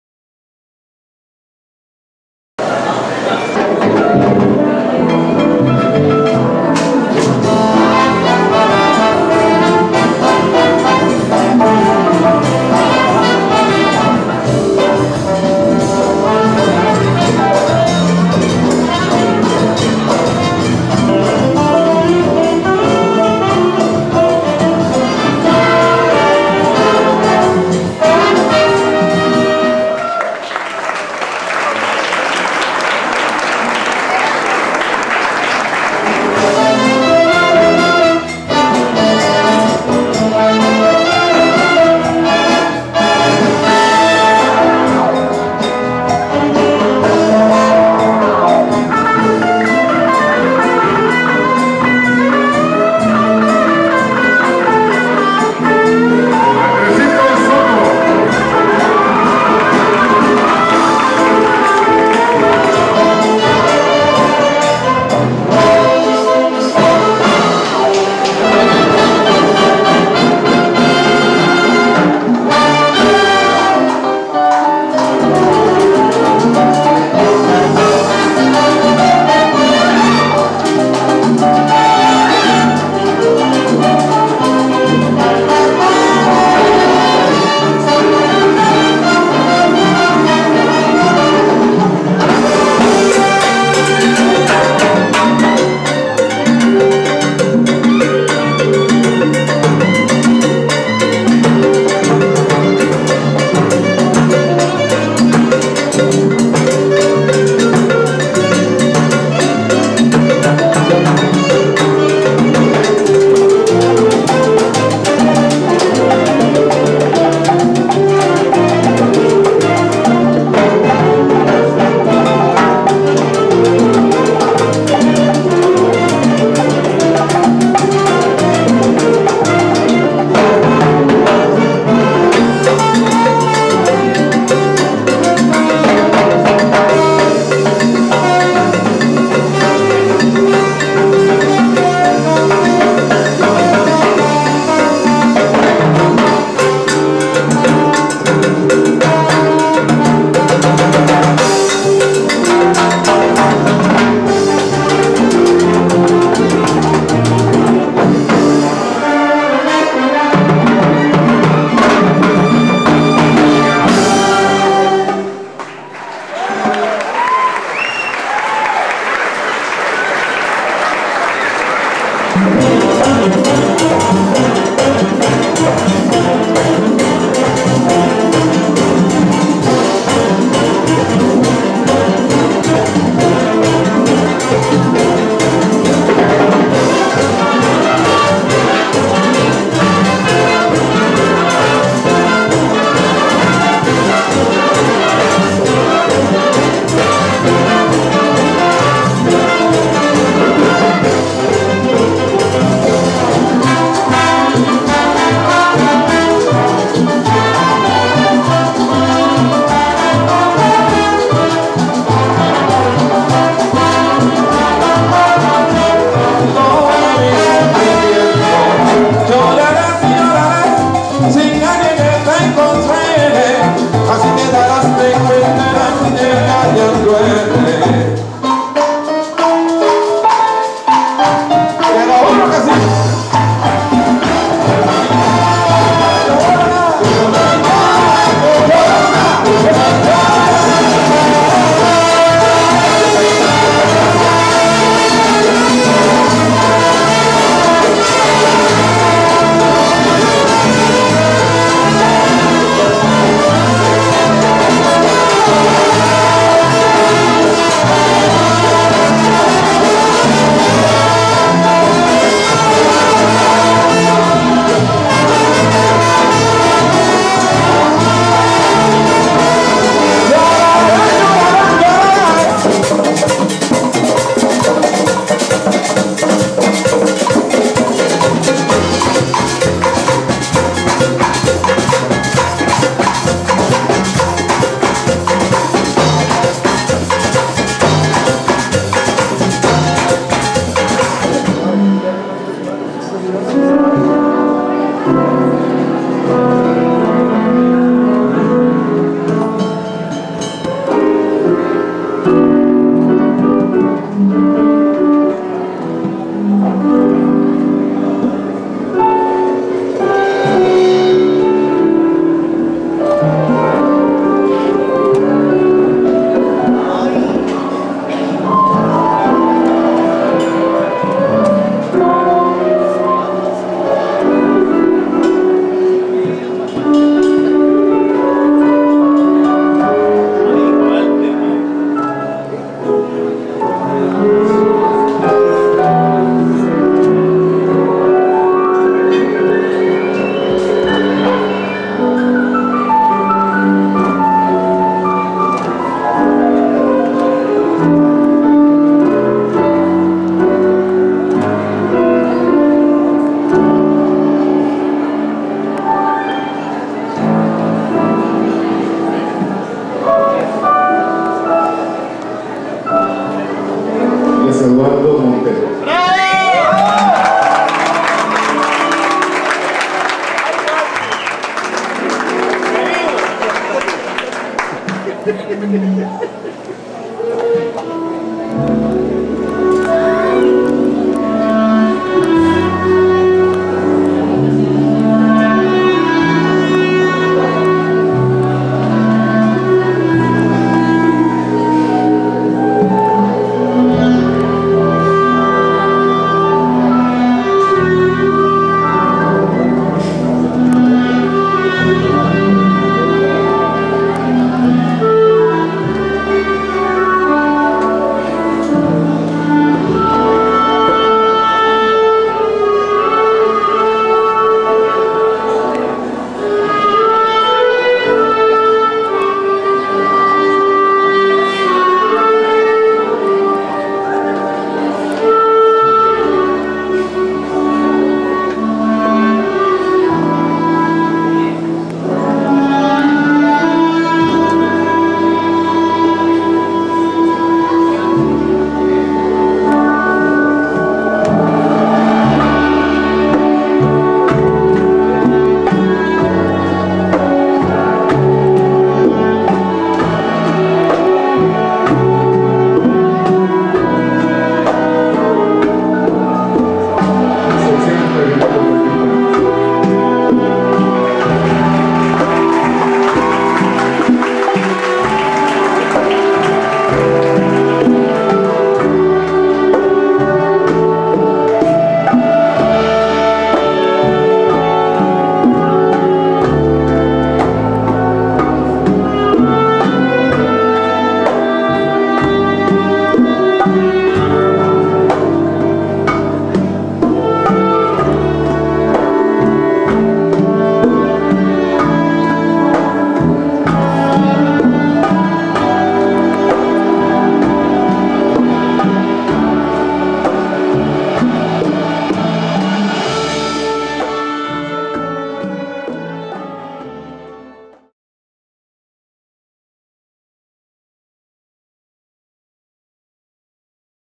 BIG BAND DE PEREZ ZELEDON - 2008 - Dirige Leo Rodriguez
LA BIG BAND DE LA ESCUELA DE MUSICA SINFONICA DE PEREZ ZELEDON CON SON DE TIKIZIA INVITADOS.